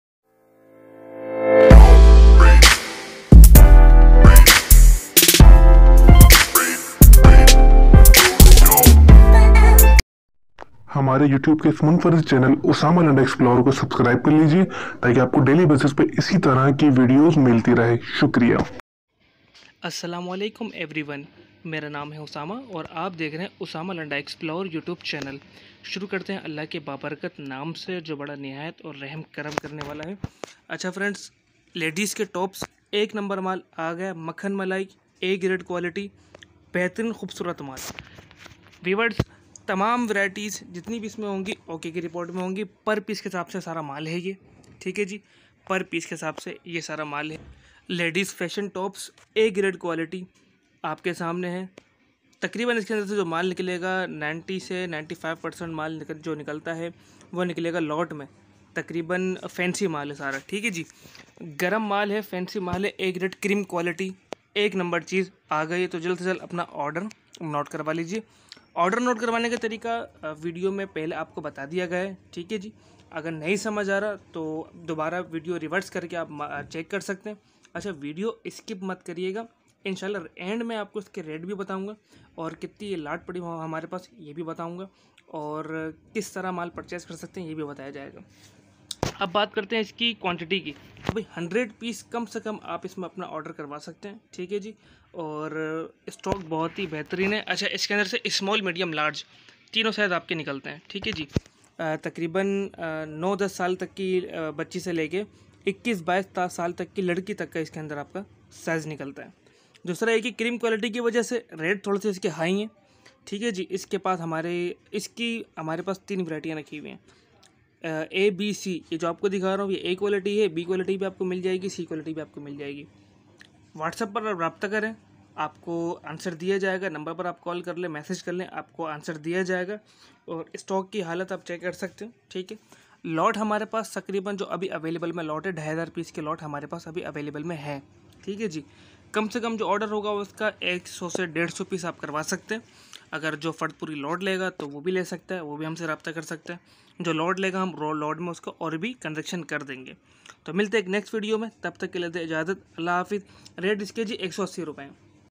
Ladies Lundon Western Winter Top | Imported Winter Tops | Winter Dresses
Today I Will Show You Ladies Western Winter Tops Ai Quwalty A Gread Lot Ladies Winter Top Wholesale Price And Cheepest Rate ..

Thankyou For Watching My.Vidio